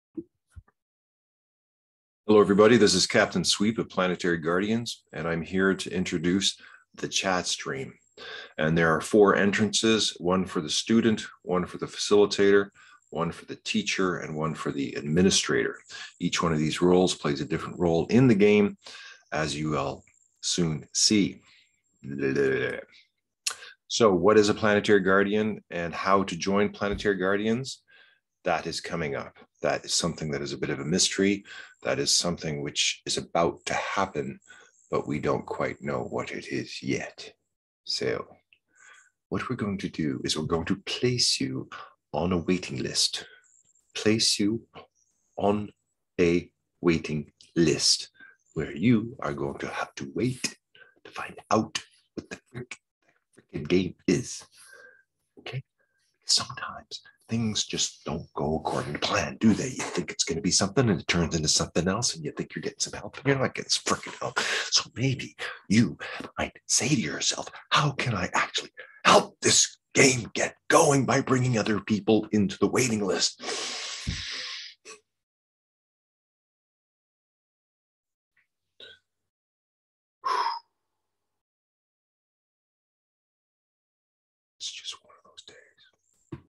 WELCOMING to the Chat Stream
Captain sweep's first attempt to explain the Planetary Guardians Chat Stream Game.